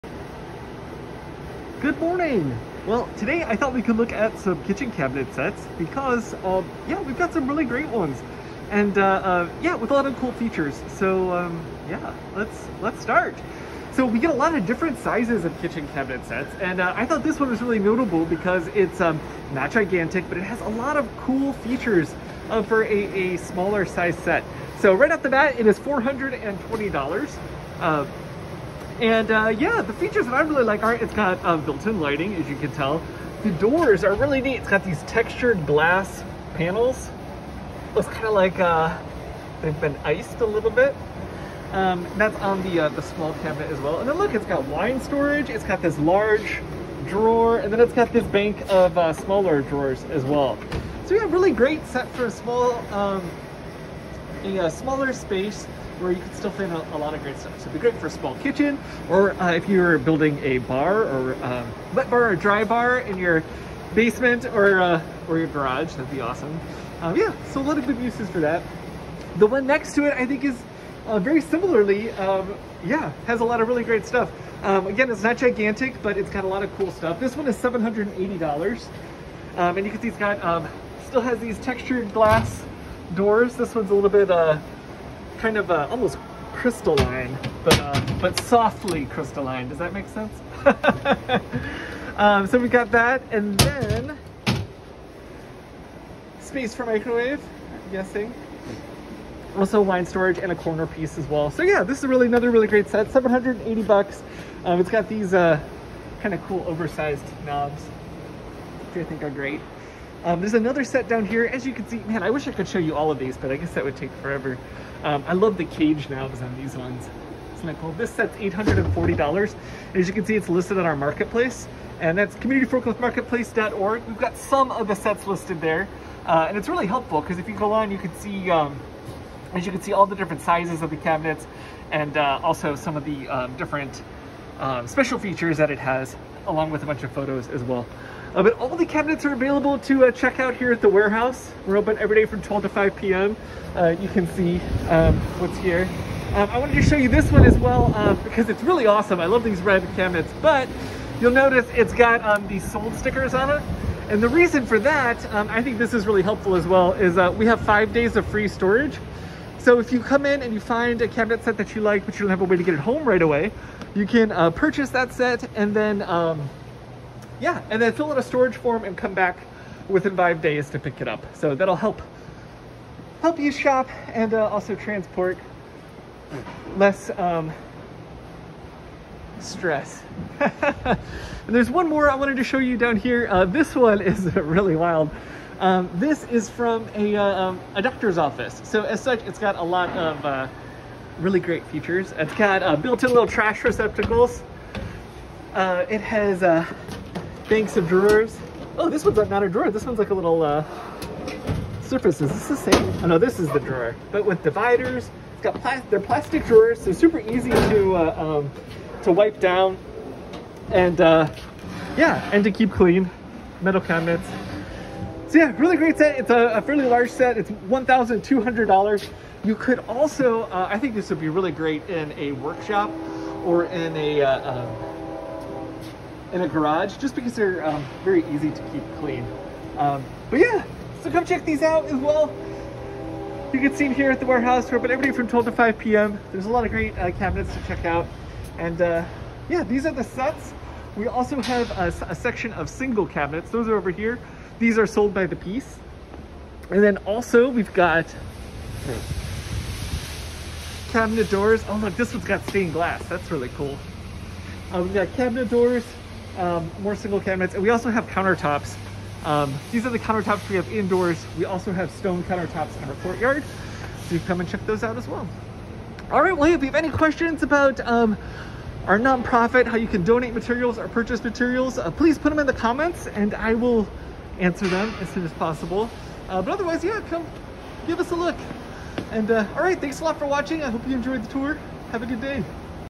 Cabinet sets for kitchen, garage, or workshop
There are a bunch of cool cabinet sets at the reuse warehouse right now, including a small set that packs in a lot of great features and a doctor's office set that's incredibly functional, as well as single cabinets and cabinet doors.